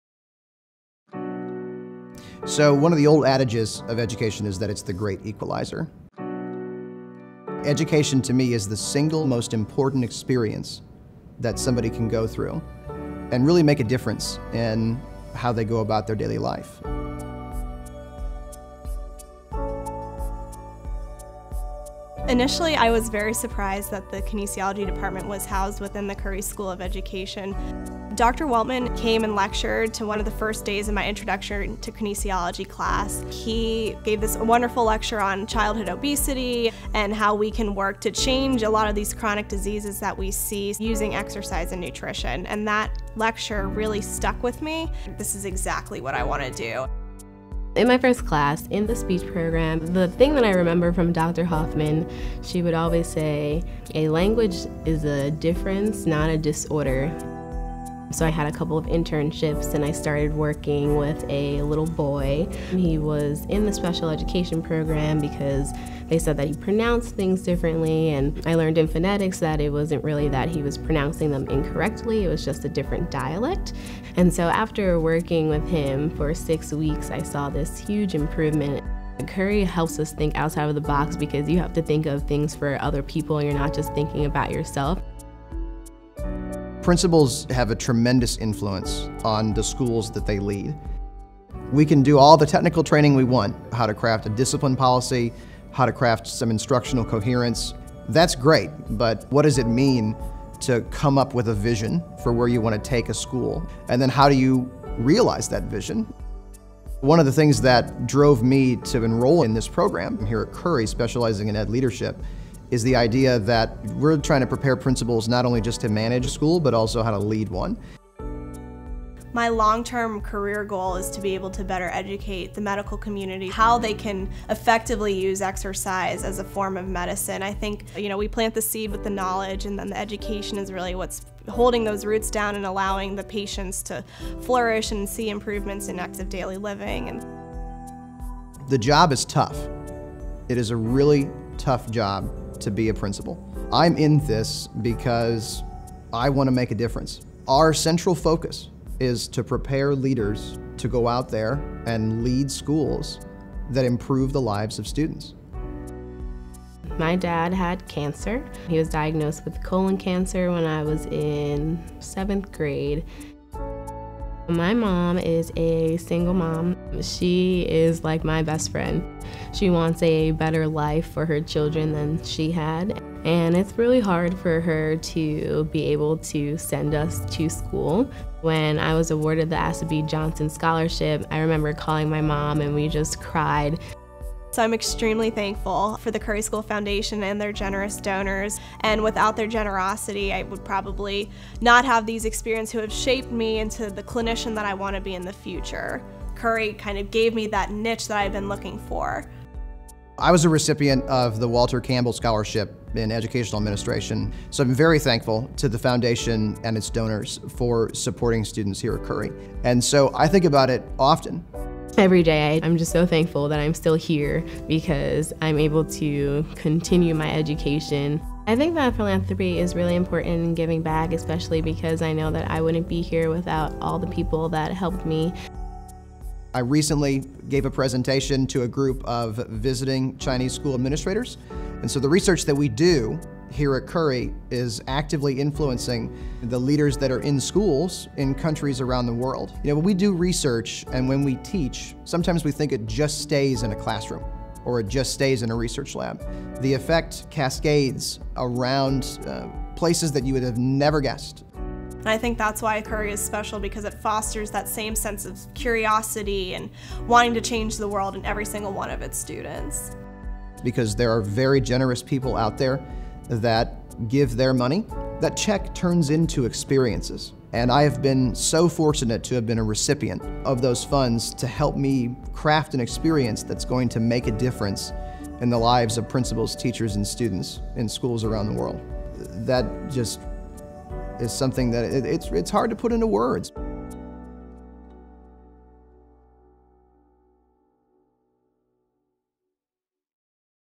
I am an Educator: The Power of Philanthropy
Philanthropy powers the work and research of 3 different Curry School of Education students.